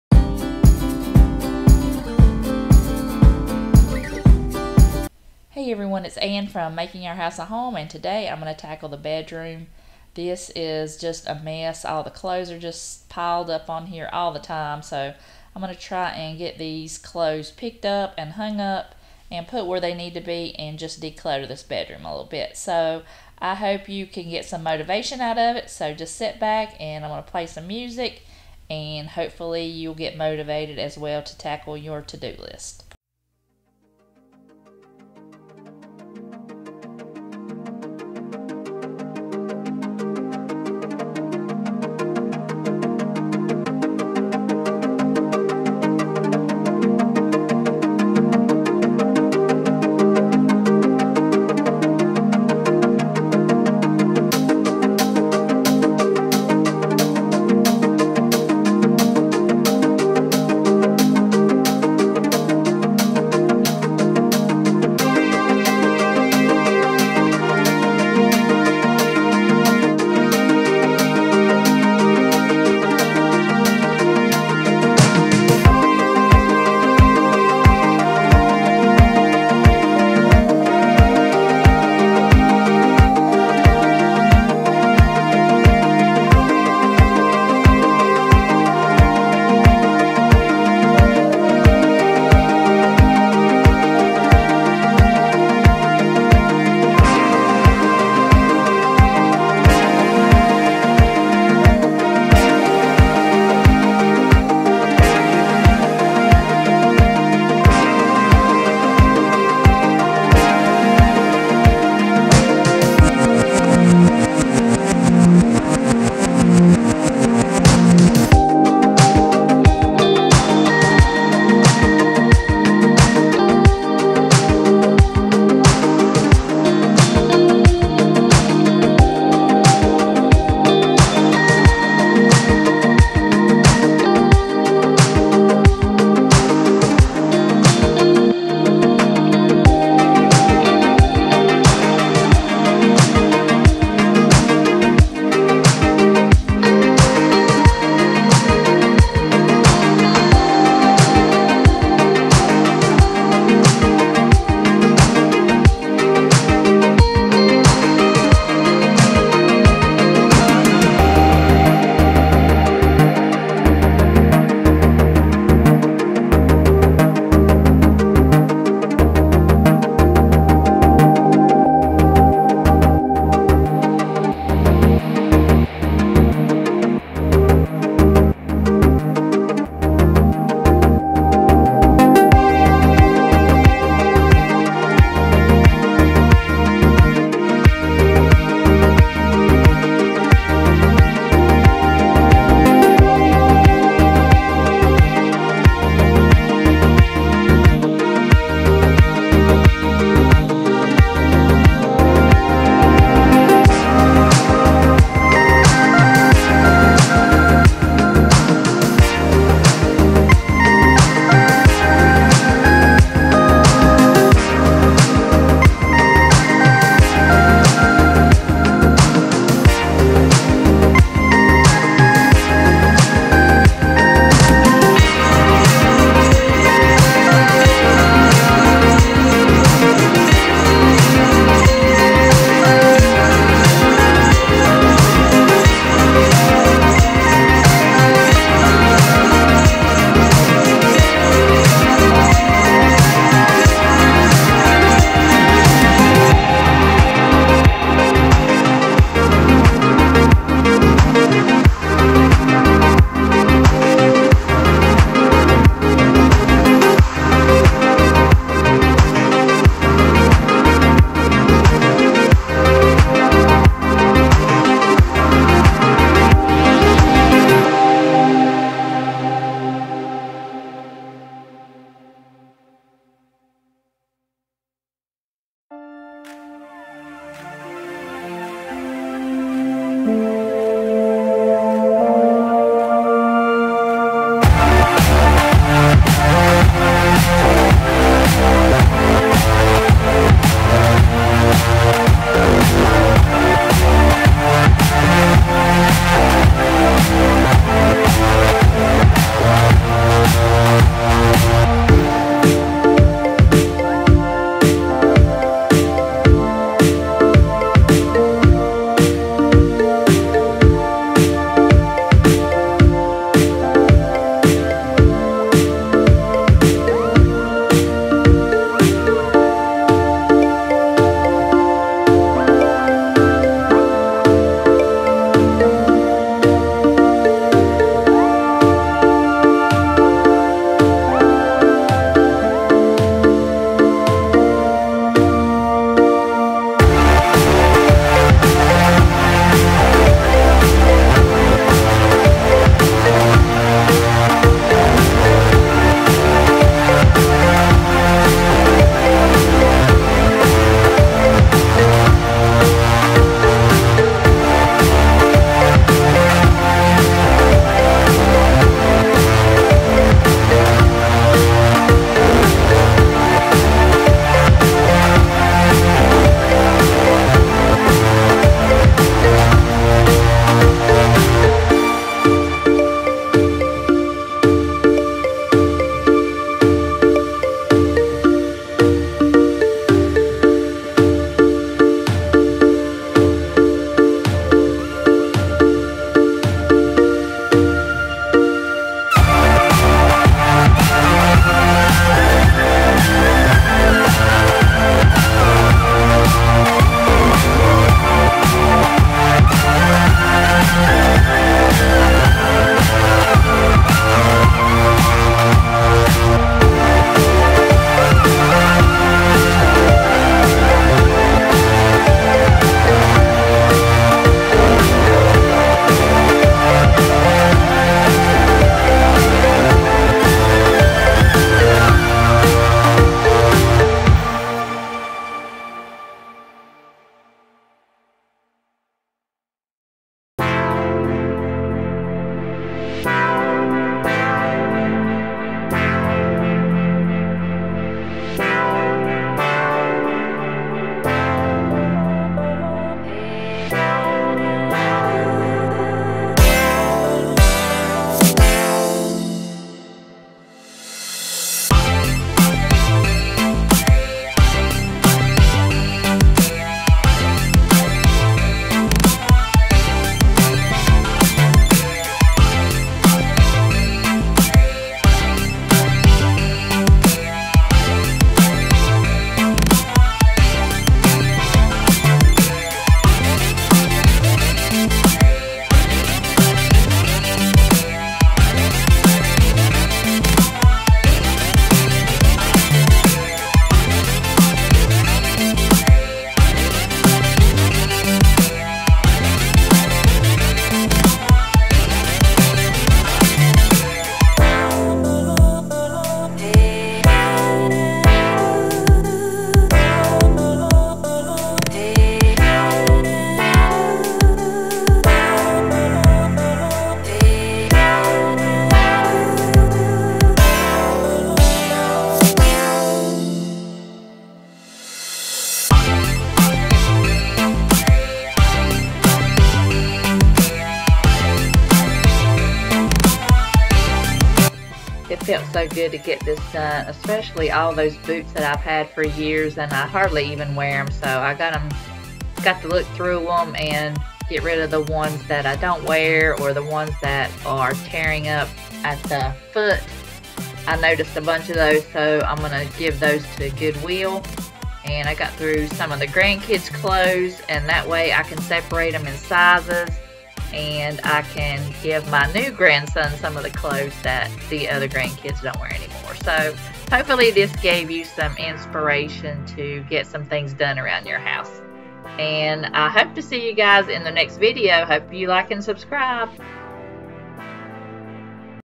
Messy Bedroom Clean! So Many Clothes! Dresser is piled full! So many kid clothes! Getting It Done!
Hey everyone! I have another cleaning video! I'm getting my bedroom clean and I'm getting the kid clothes decluttered and going through my winter boots! So many kids clothes! I hope you get some cleaning motivation! I'm so glad to get this done! Do you leave all your clothes piled up on your dresser? I love hearing from you!